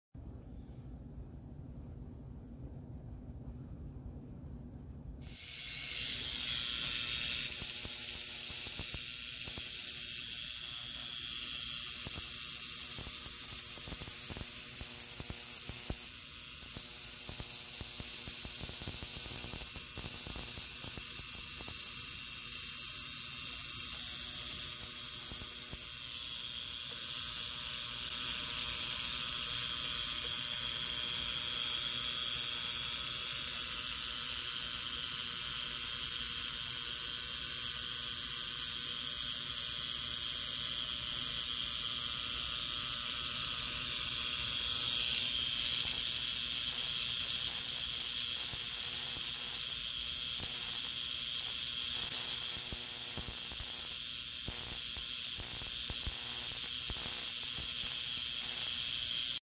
DEM59, Tesla Coil with Accessories
Setup & use of DEM59, Tesla Coil with Accessories